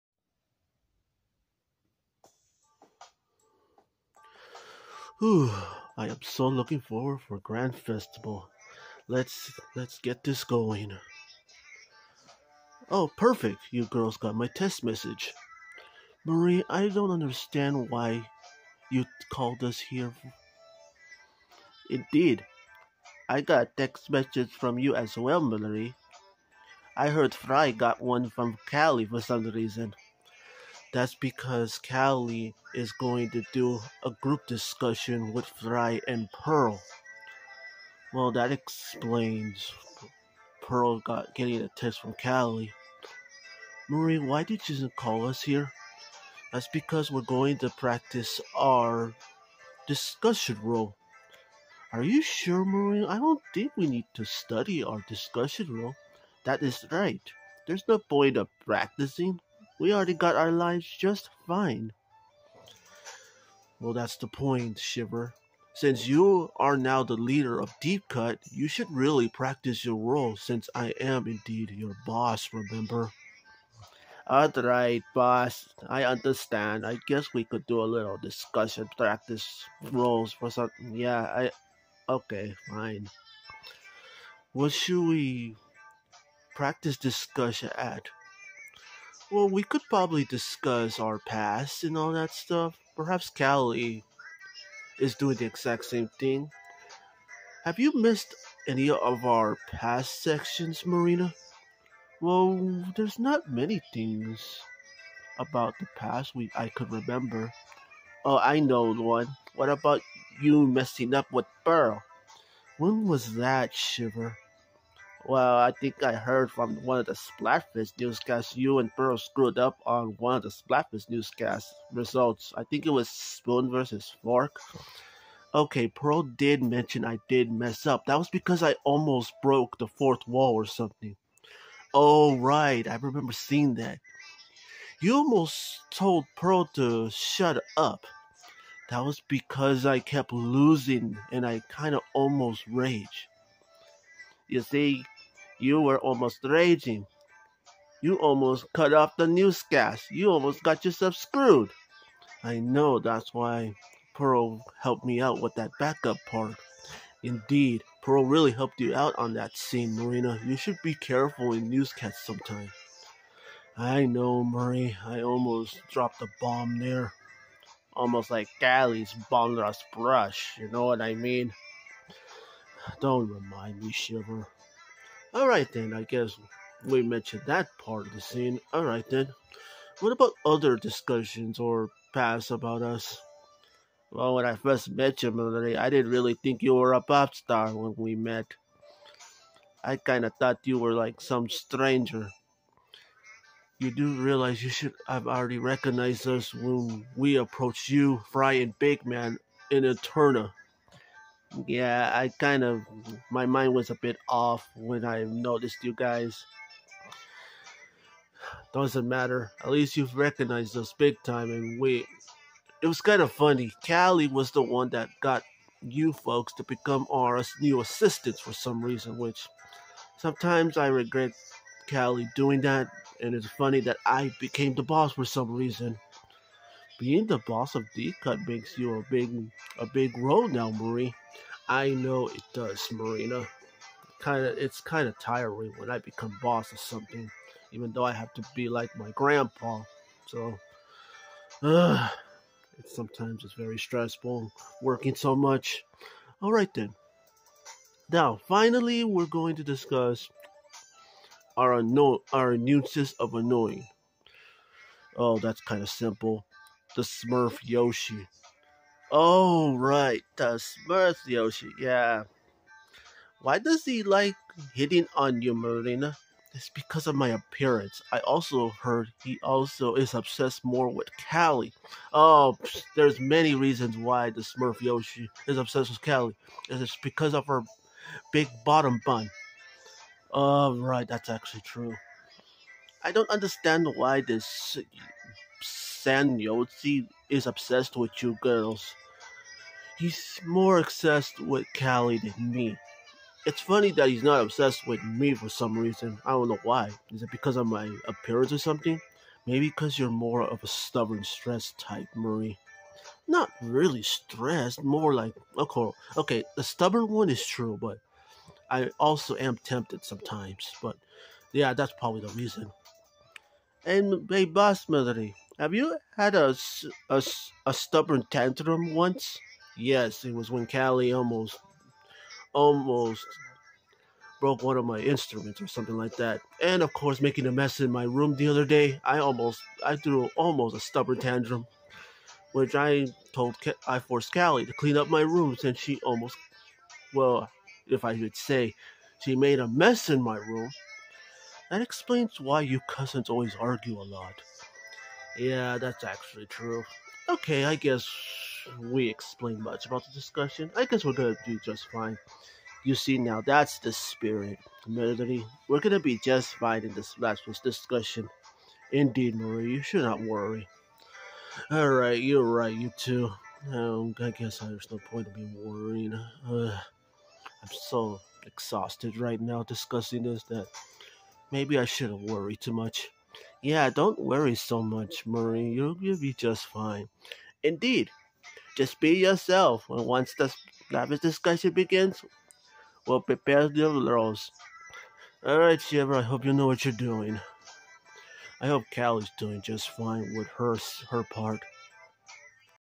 Splatoon Plush Grand Festival Edition Part 2/2
Marie invites Marina and Shiver to a practice discussion session.

The gals discuss a few stuffs like Callie, Pearl and Frye.

Enjoy Part 2 of this plush video peeps.